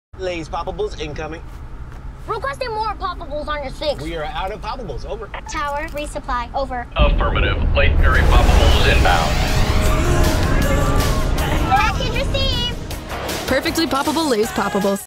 Lay’s Poppables | Co-Pilot :15
When your snack’s more fun, everything’s more fun. Don’t hit the road without perfectly poppable Lay’s Poppables.